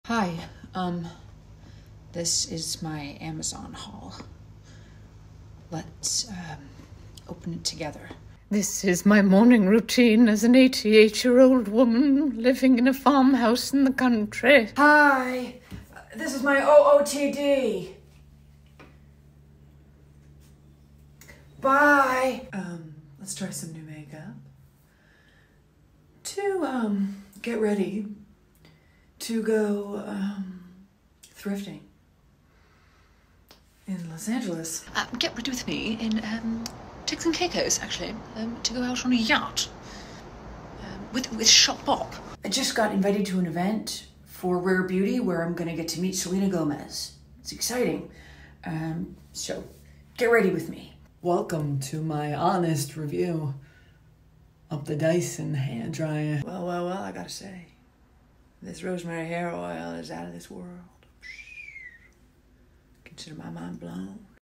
celebs as influencers 🤩
if these famous actors were social media influencers. i'd honestly pay money to see dame maggie's morning routine.

Original celebs as influencers video by Mary Elizabeth Kelly (me) with impressions of Winona Ryder, Dame Maggie Smith, Jennifer Coolidge, Meryl Streep, Dame Emma Thompson, Jodi Foster, Christopher Walken, & Matthew McConaughey.